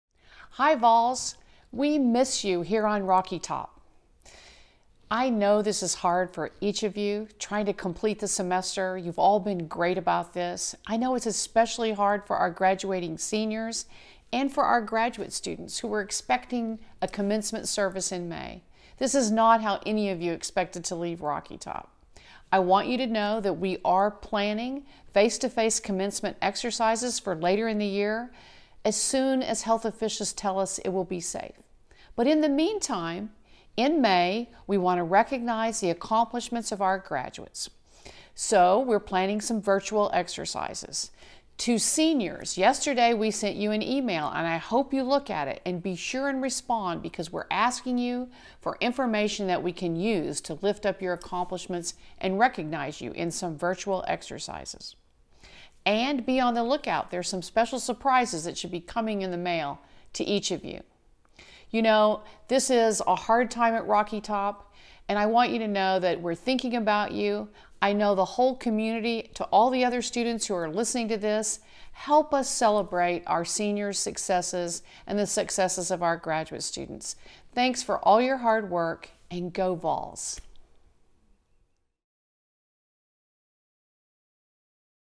Chancellor's Weekly Message to Students—April 16, 2020
University of Tennessee, Knoxville, Chancellor Donde Plowman encourages students to finish the spring semester strong and shares the excitement around plans to recognize accomplishments of our graduates.